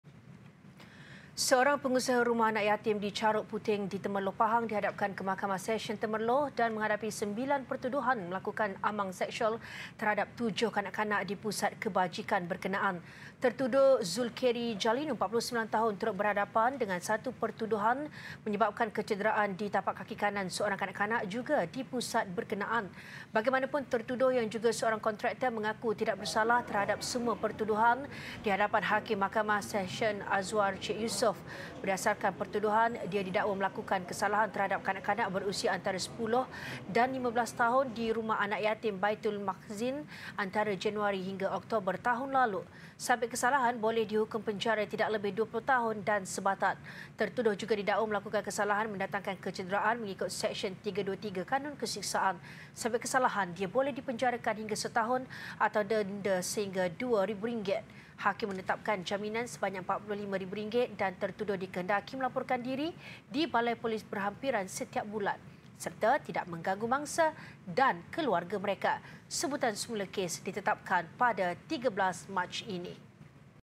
Pengusaha Rumah Anak Yatim Hadapi 10 Pertuduhan
Seorang pengusaha Rumah Anak Yatim di Charuk Puting di Temerloh, Pahang dihadapkan ke Mahkamah Sesyen Temerloh dan menghadapi  9 pertuduhan melakukan amang seksual terhadap 7 kanak-kanak di pusat kebajikan berkenaan. Tertuduh, Zulkeri Jalinu, 49 tahun turut berhadapan dengan satu pertuduhan menyebabkan kecederaan di tapak kaki kanan seorang kanak-kanak juga di pusat berkenaan.